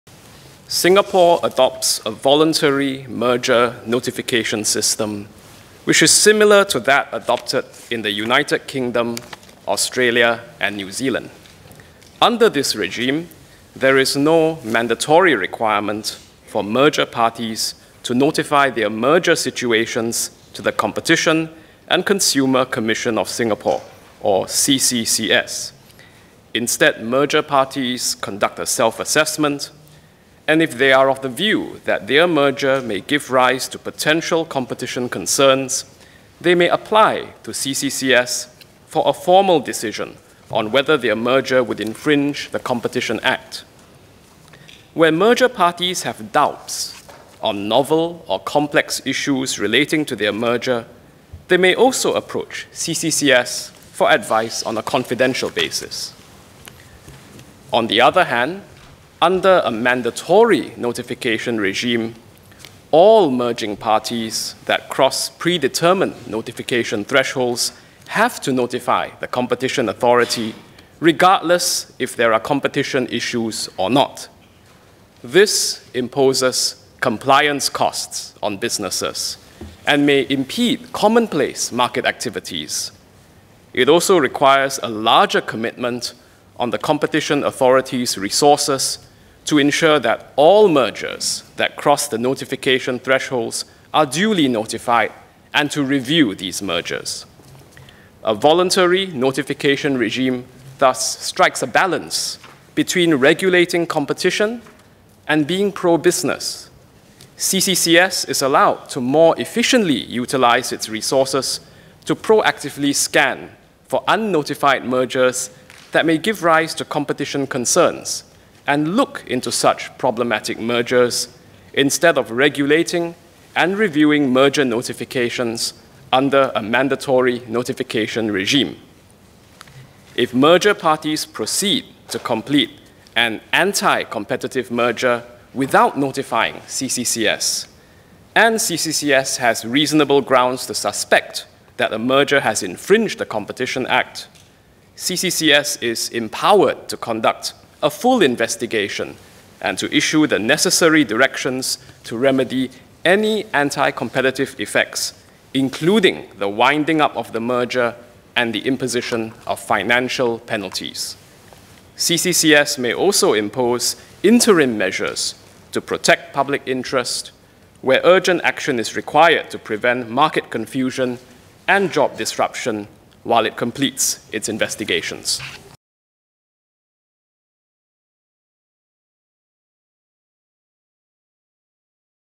SPS Tan Wu Meng: Regulating business competitiveness in Singapore
He spoke on the balance that the Competition and Consumer Commission (CCCS) maintains, between regulating competition and being pro-business.